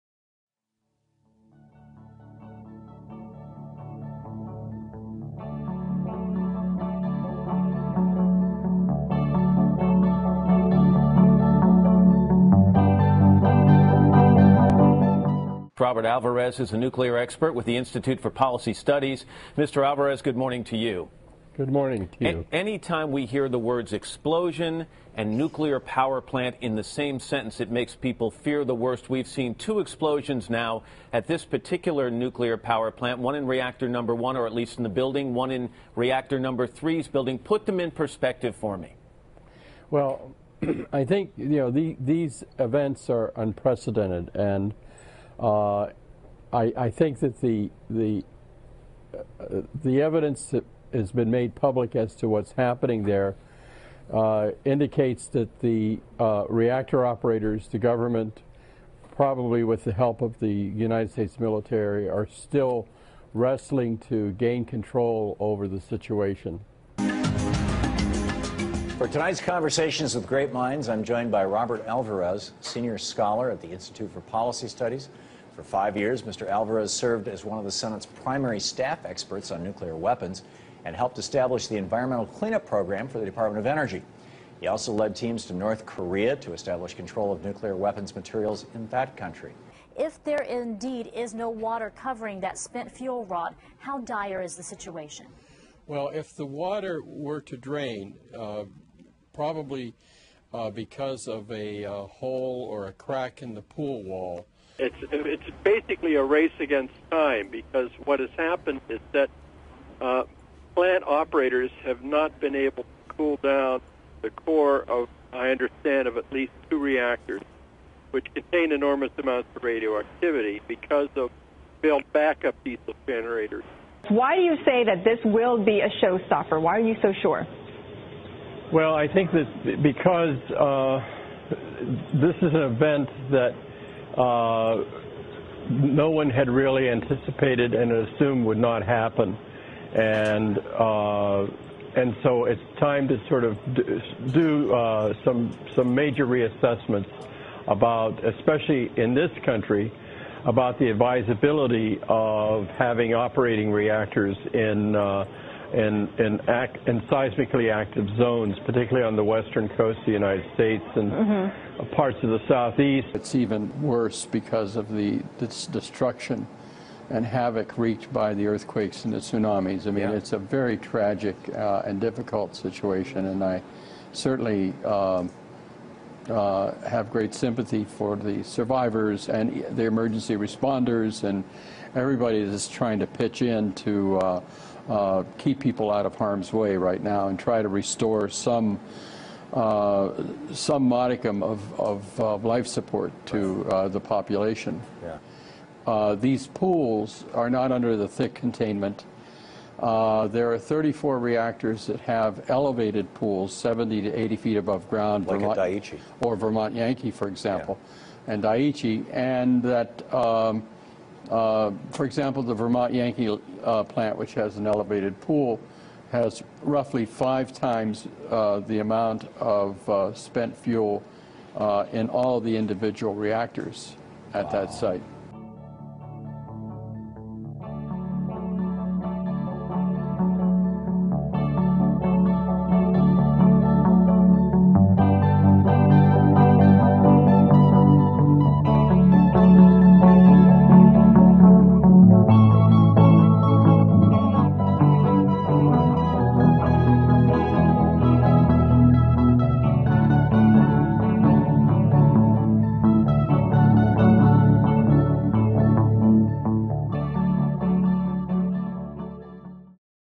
Nuclear Catastrophe: Robert Alvarez's Media Appearances post-Fukushima
In the wake of the nuclear disaster in Fukushima, IPS expert Robert Alvarez was a critical player in the media, keeping the U.S. public informed about the status of the reactors and the public health risks.